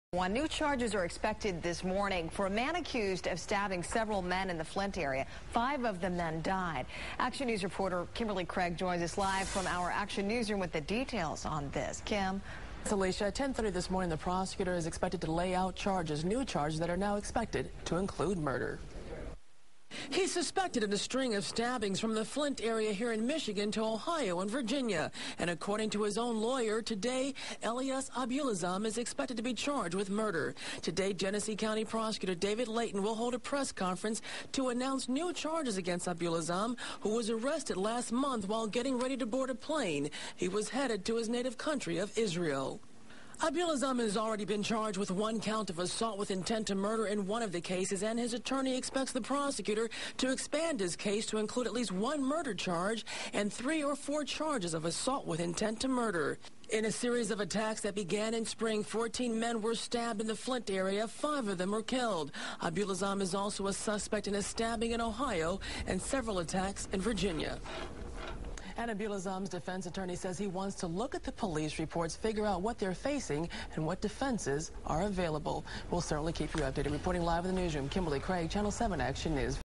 More charges expected in serial stabbing case
More charges are expected to be filed against suspected serial stabber.